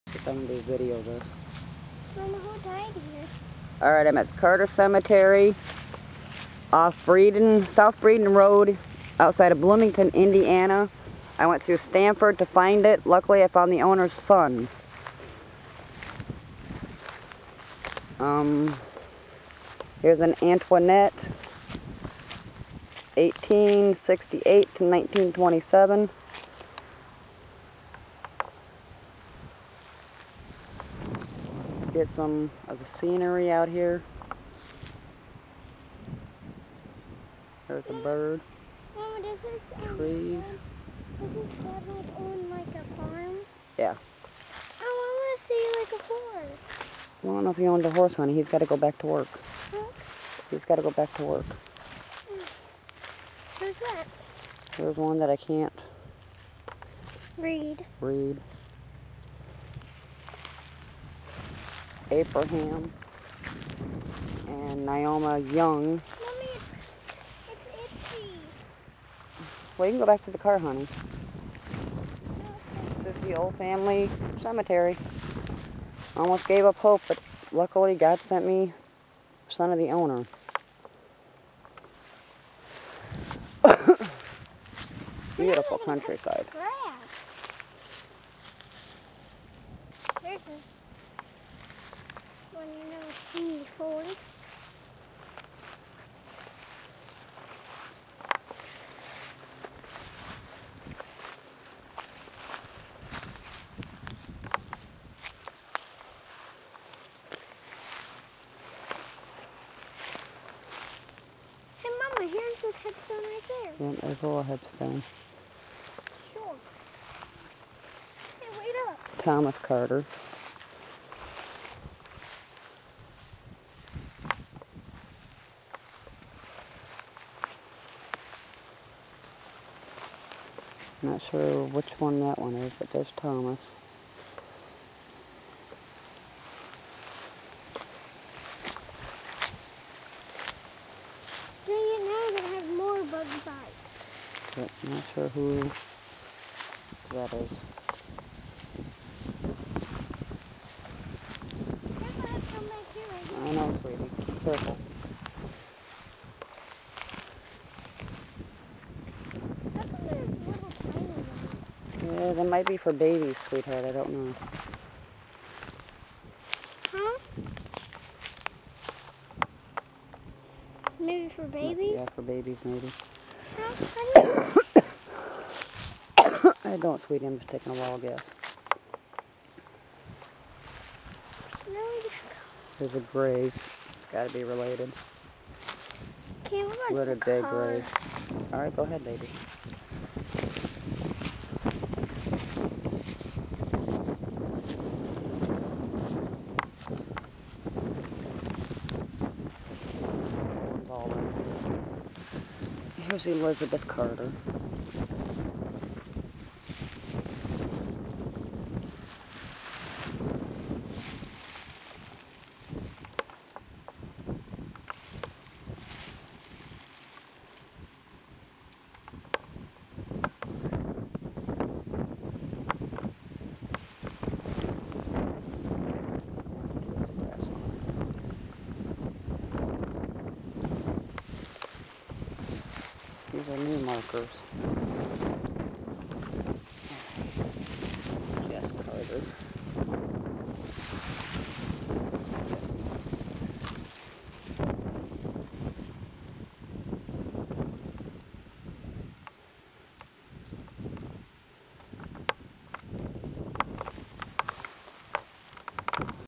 carter cemetery off of south breeden road south of bloomington indiana.right after stanford in.
thomas carter is my moms gr.gr. grandfather.south breeden road turns into west carter. do not turn where you see the west carter sign.look for 2 white house with alot of space in between. the owners son works somewhere in there. gmc pick up 2 tone red and gray. rust on drivers side rear corner panel over rear tire.